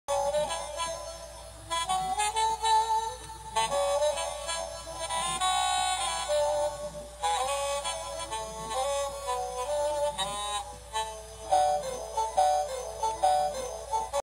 Chinafake Animated Festal Little Man (Saxophone Version)
Made by an unknown company. Bought from Ebay US along with a violin variant. It has a B&D Box layout but different design, very very similar to the Jazzman - Erik The Fish Guy